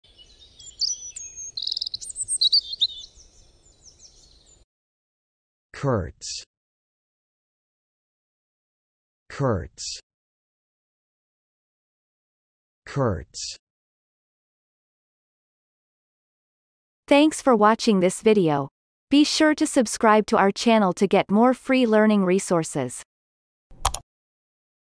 How to pronounce kurtz - Vocab Today
The pronunciation of the word kurtz in Amercian accent is demonstrated in this video. If the word is from another language, such as brand name, it will be pronounced in its original language. Based on the word frequency, this is of 4 stars difficulty.